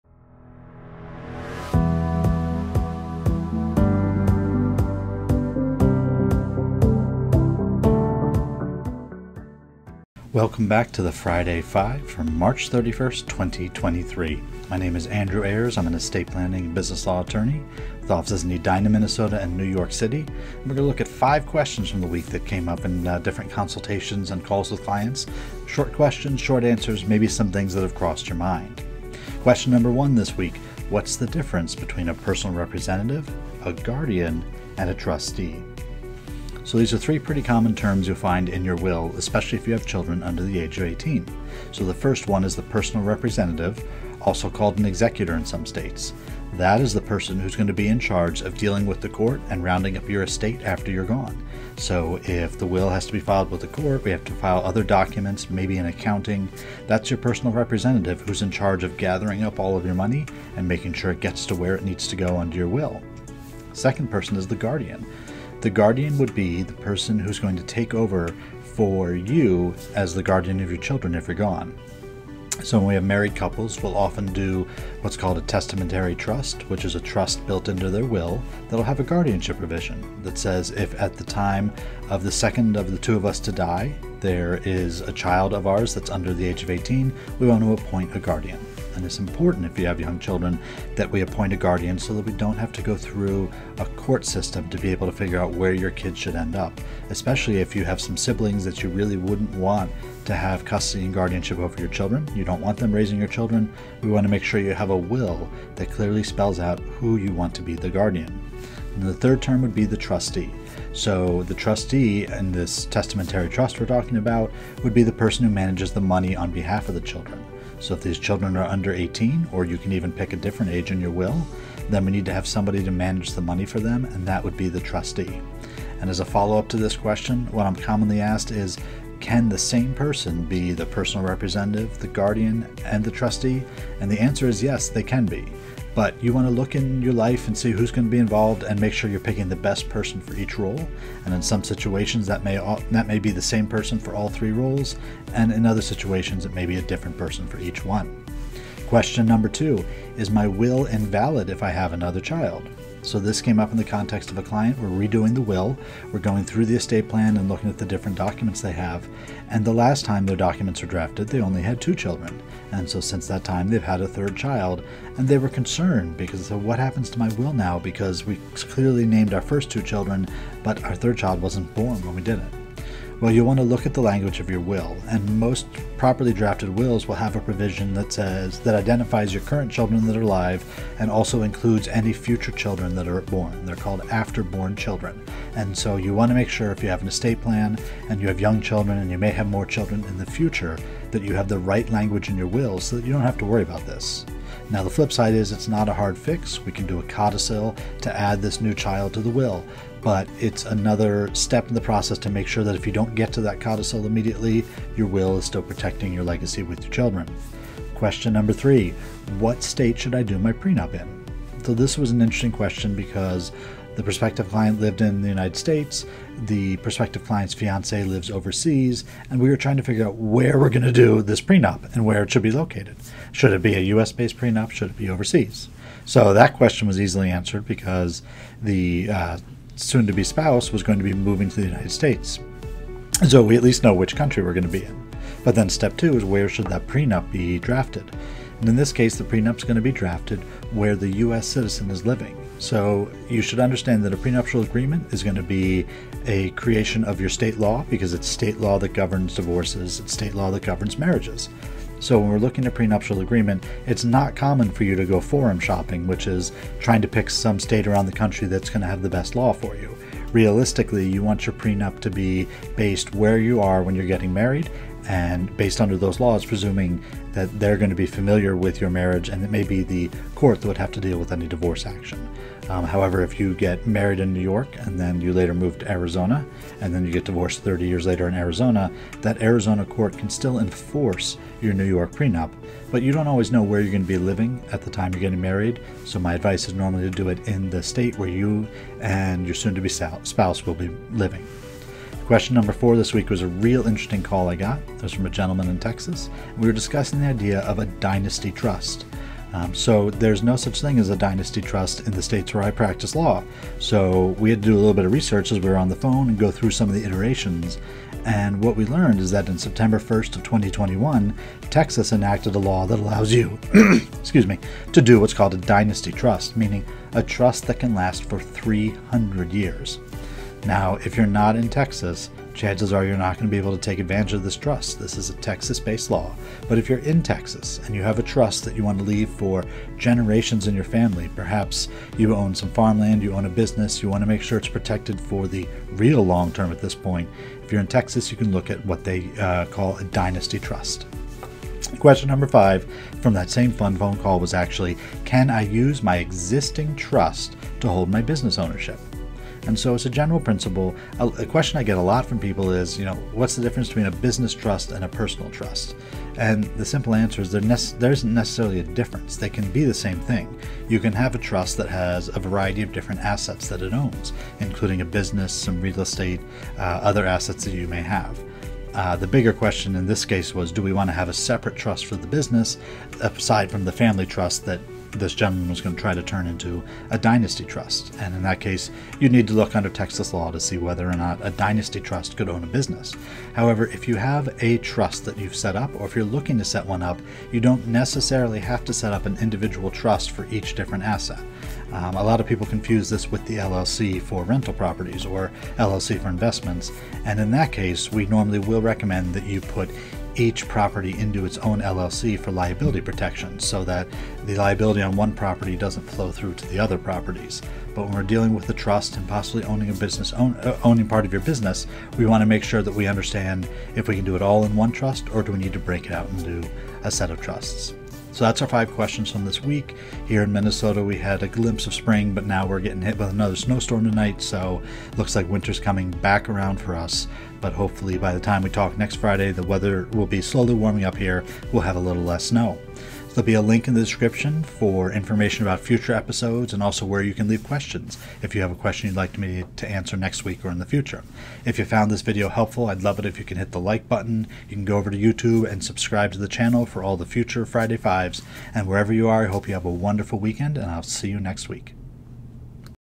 Friday 5 - March 31, 2023
We've had a short preview of spring this week, but there's more snow on its way tonight to Minnesota. In this week's Friday 5 we look at some estate planning issues, a prenup question and some interesting trust questions:

❓ What is the difference between a personal representative, guardian and trustee?
❓ Is my will invalid if I have another child?
❓ What state should I do my prenup in?
❓ What is a Dynasty Trust?
❓ Can I use my existing trust to hold my business ownership?

Submit your question to be featured in my Friday 5 video series

00:00 Friday 5 for March 31, 2023
00:29 What is the difference between a personal representative, guardian and trustee?
02:29 Is my will invalid if I have another child?
03:27 What state should I do my prenup in?
04:51 What is a Dynasty Trust?
05:41 Can I use my existing trust to hold my business ownership?